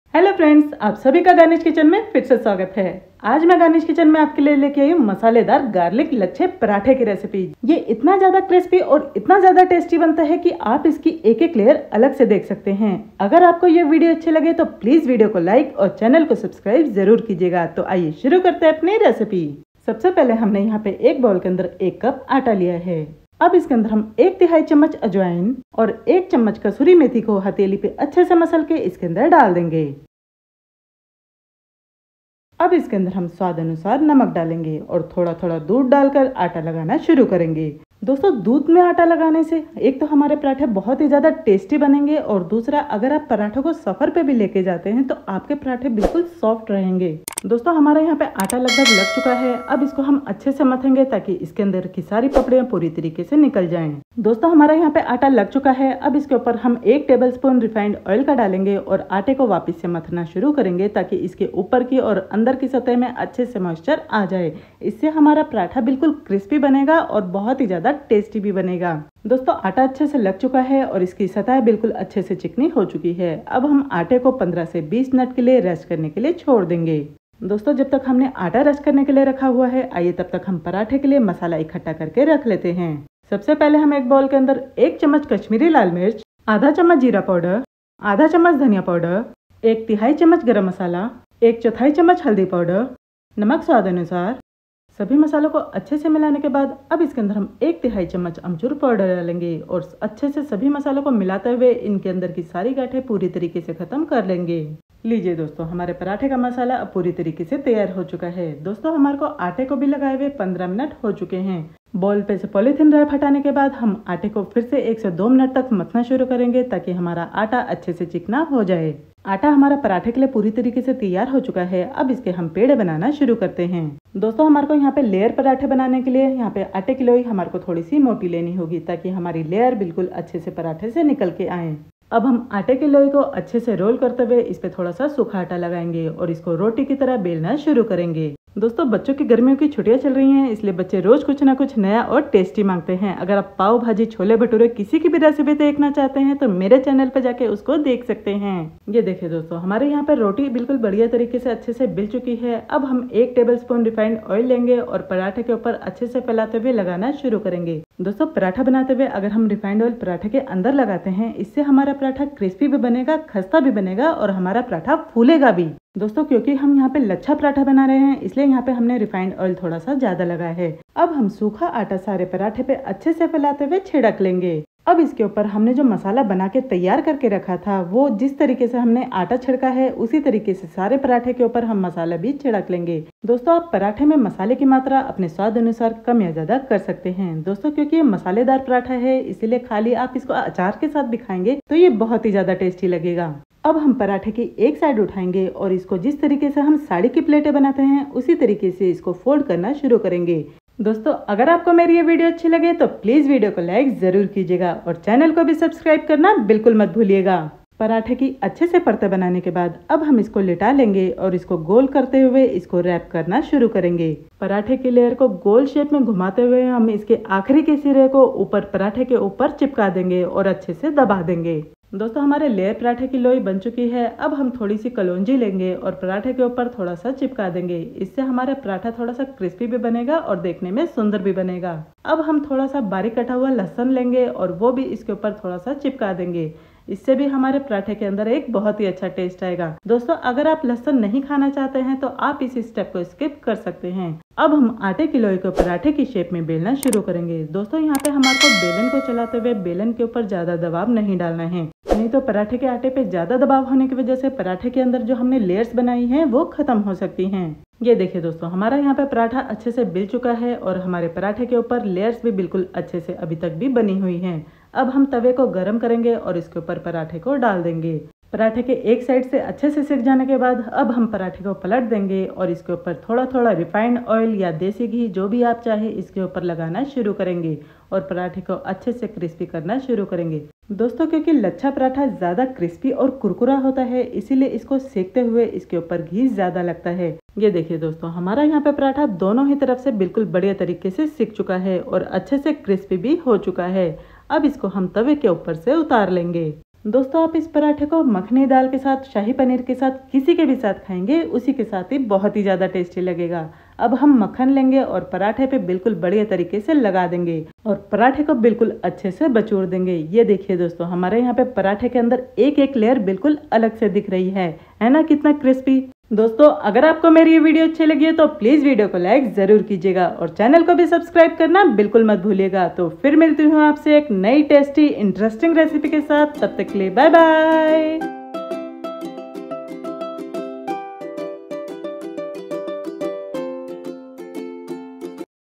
Laccha Paratha | Multi Layer Laccha Paratha Recipe | लच्छा पराठा बनाने की आसान विधि
0:17 Intro

Ingredients :
For Dough

1 Cup Wheat Flour

1/3 Tsp Carom Seeds

1 Tsp Kasoori Methi

Milk

Refined Oil

For masala :

1 tsp kashmiri red chilli powder

1/2 Tsp Cumin Seeds Powder

1/2 Tsp Coriander Powder

1/3 Tsp Garam Masala

1/4 Tsp Turmeric Powder

1/3 Tsp Amchur Powder

Salt
For taste:

Butter